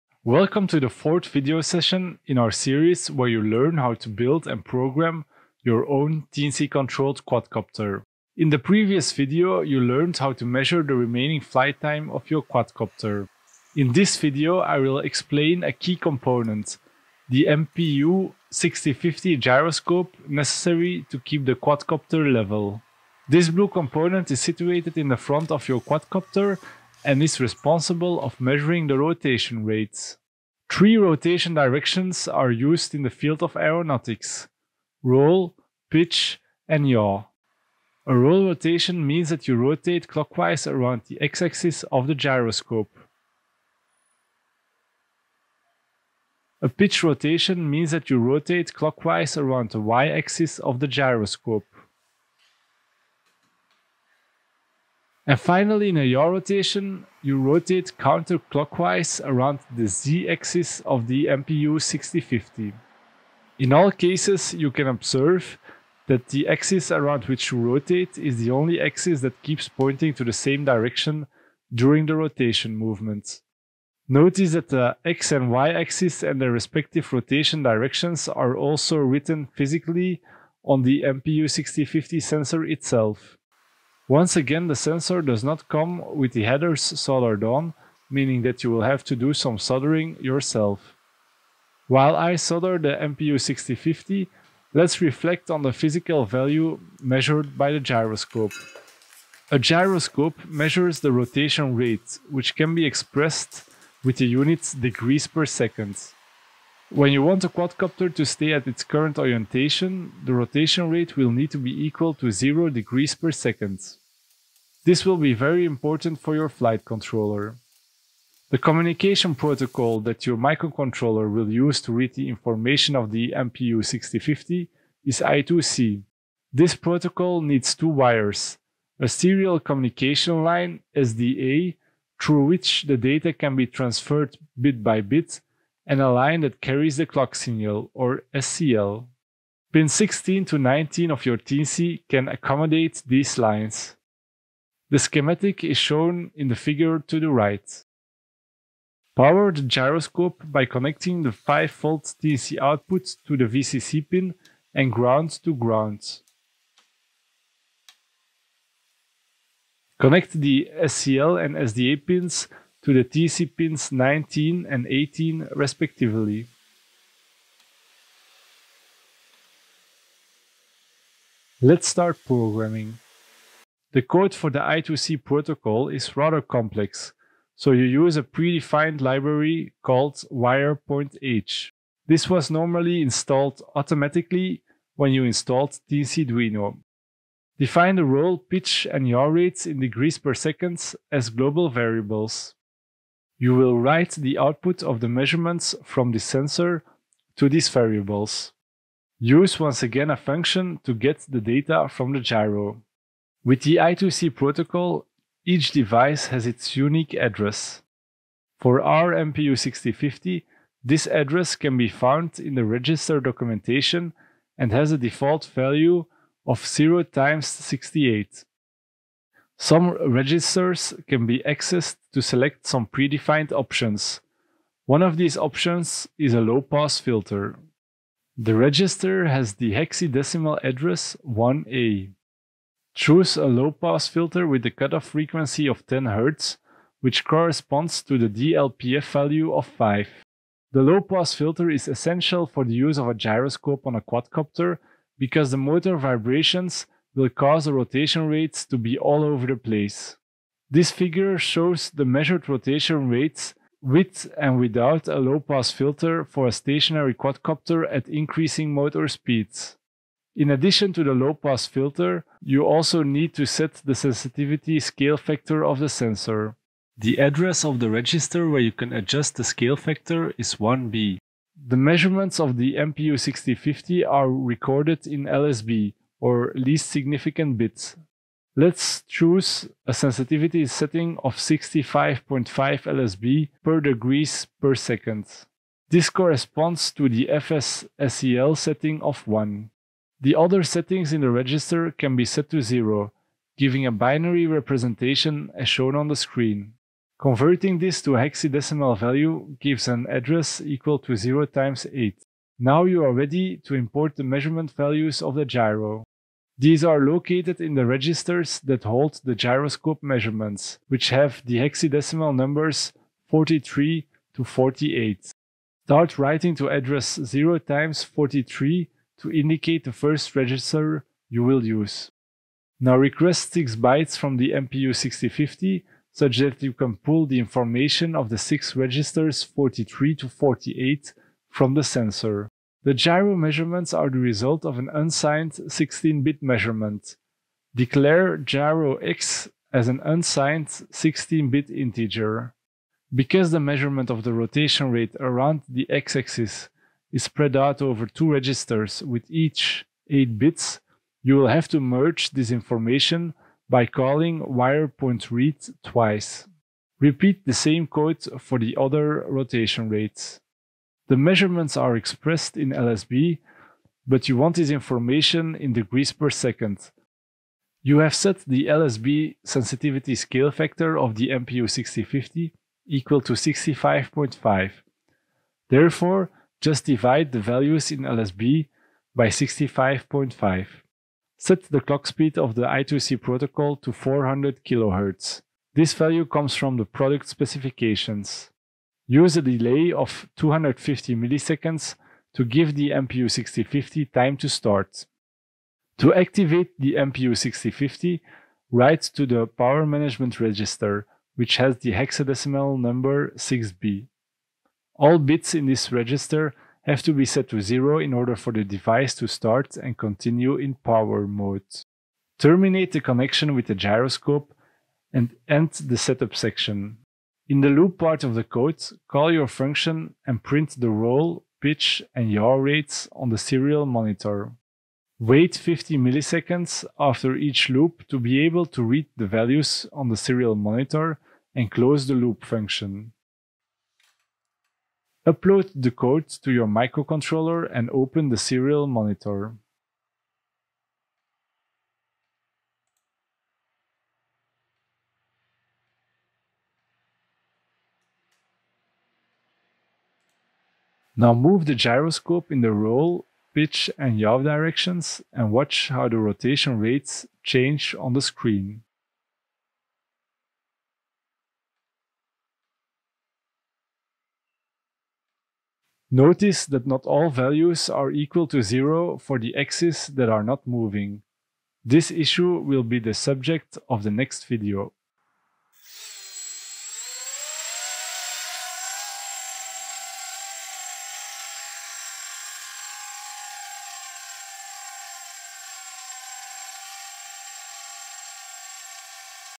4 | How to use the MPU6050 with Arduino and Teensy
This video explains how to use the MPU-6050 for the measurement of the roll, pitch and yaw rotation rates. You will learn how to connect the GY-521 breakout board to your Teensy 4.0 microcontroller and how to access the rotation rate measurements.

The purpose of this video series is to learn the basics behind a quadcopter drone and enable you to build one yourself, by dividing this challenging project in several easy-to-understand parts. You use the capable Teensy 4.0 microcontroller together with the easy-to-use Arduino language.